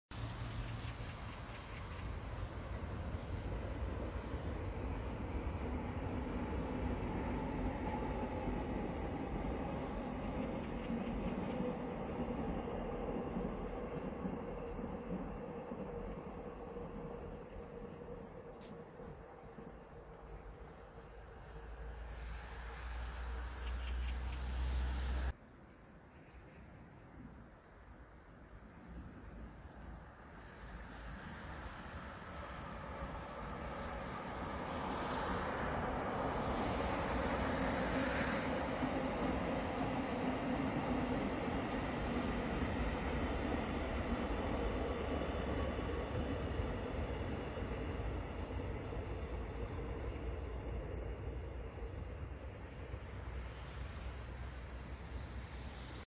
경부선, 2월 10일 눈내린 날에 어울리는 무궁화호
경북 김천시 대항면  직지사 굴다리 위를 지나는 무궁화호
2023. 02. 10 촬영